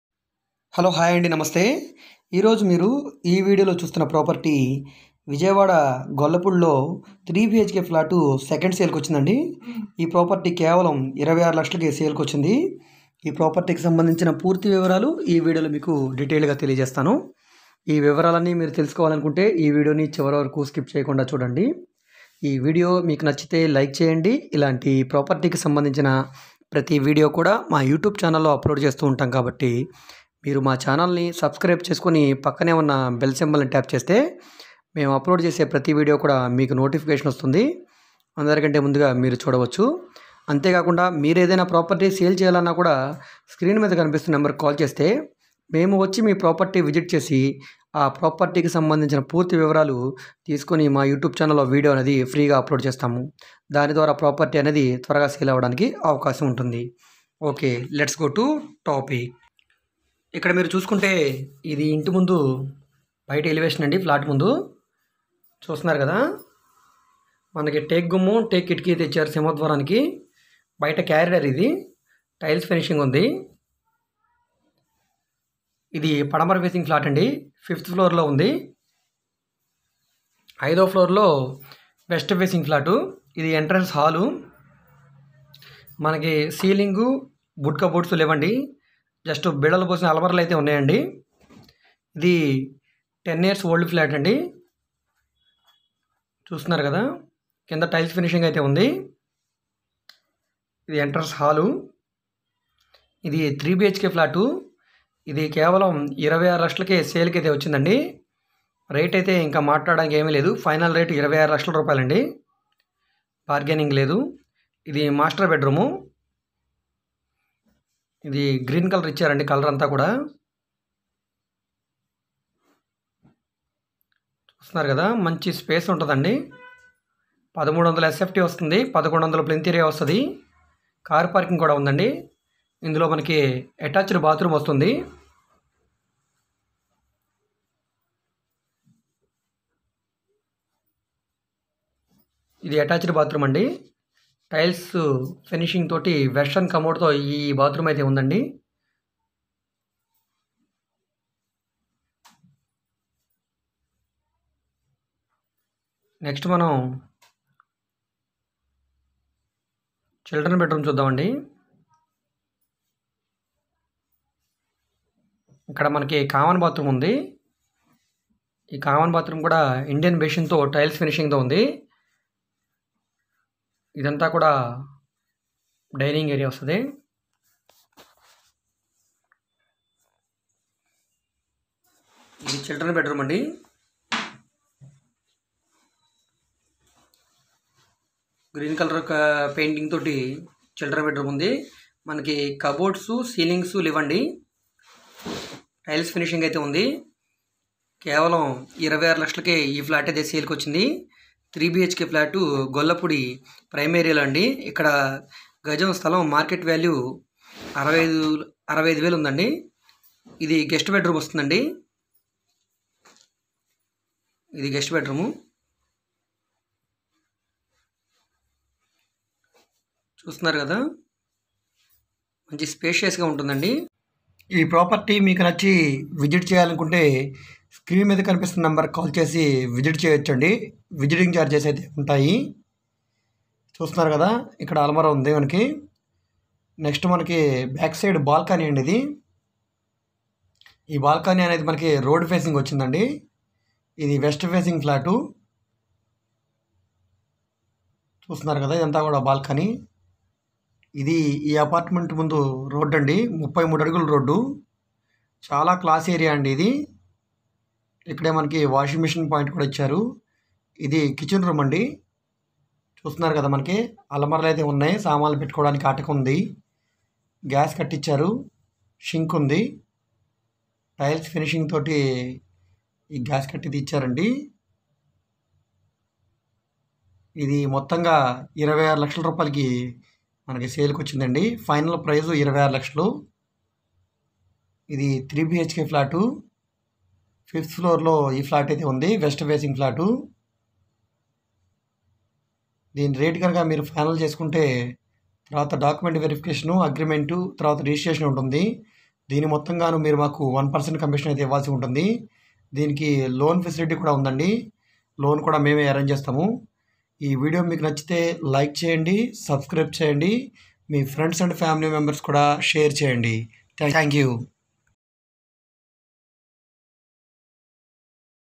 26lak 3bhk flat sale in vijayawada gollapudi || low budget house sale in vijayawada || rental house
విజయవాడ గొల్లపూడిలో కేవలం26లక్షలకే 3bhk ఫ్లాట్ సేల్ కి వచ్చింది కావాల్సిన వారు సంప్రదించండి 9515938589